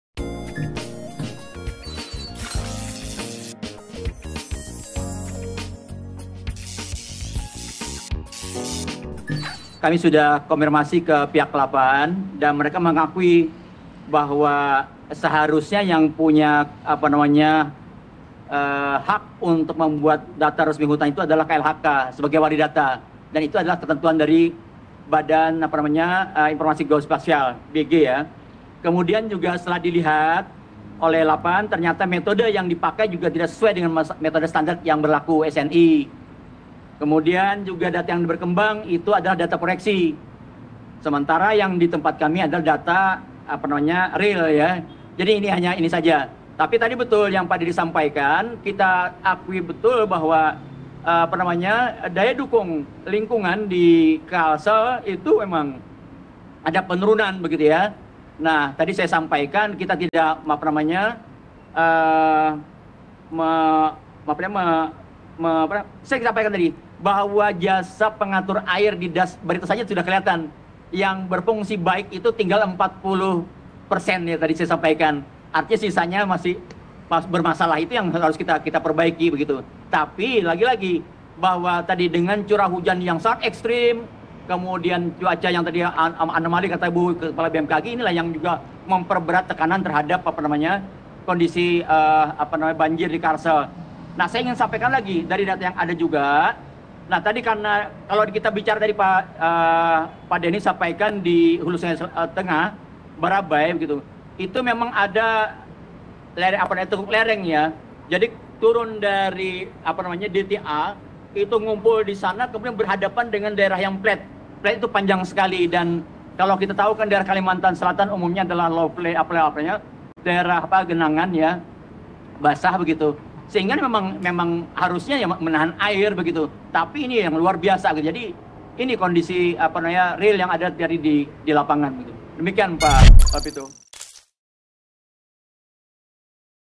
KLHK Sebut Lapan Tak Ada Wewenang Rilis Data Luas Hutan Kalsel!
Kementerian Lingkungan Hidup dan Kehutanan menampik data Lapan terkait luas lahan hutan di Kalimantan Selatan. Data disebut tidak menggunakan standar metode SNI. Berikut penjelasan Direktur Jenderal Pengendalian Pencemaran dan Kerusakan Lingkungan Kementerian LHK M.R. Karliansyah dalam D'Rooftalk (20/1).